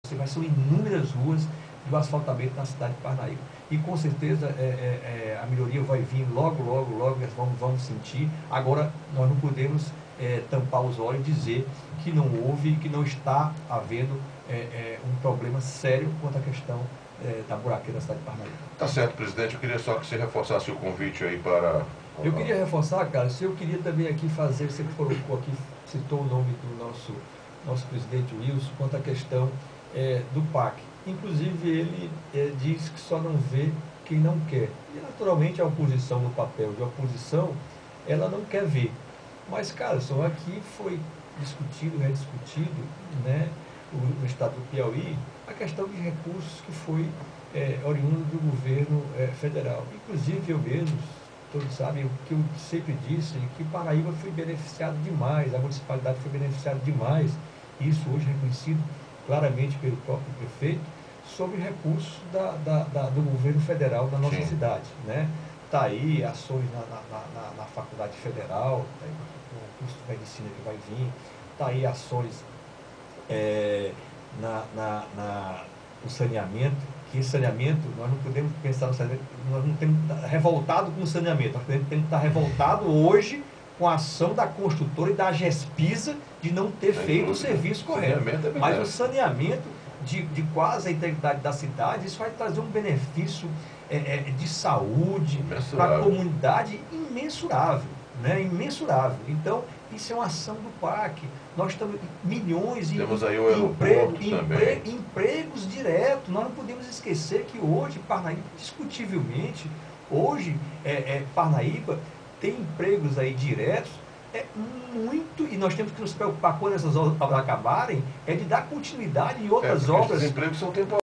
Mirocles Veras PSB coordena encontro do Partido em Parnaíba VII
Encontro do PSB em Parnaíba
Mirocles Veras (PSB) coordena encontro do Partido em Parnaíba
Partido terá candidato a governador do Piauí em 2010

O presidente do PSB de Parnaíba, Mirocles Veras foi o entrevistado de hoje do FLAGRANTE. Ele falou do encontro do partido que acontece nesta sexta feira (17) com a presença do presidente da regional do PSB, vice governador Wilson Martins. Estão sendo aguardadas representações de onze municípios de parte da região norte do estado do Piauí, além dos filiados de Parnaíba. Ele também falou da participação do PSB na gestão do atual prefeito de Parnaíba e adiantou que já esta pronta a nova reforma administrativa da Prefeitura de Parnaíba. Também falou situação caótica em que se encontra a cidade, e disse providencias já estão sendo adotadas pela Prefeitura. Veja vídeo de parte da entrevista.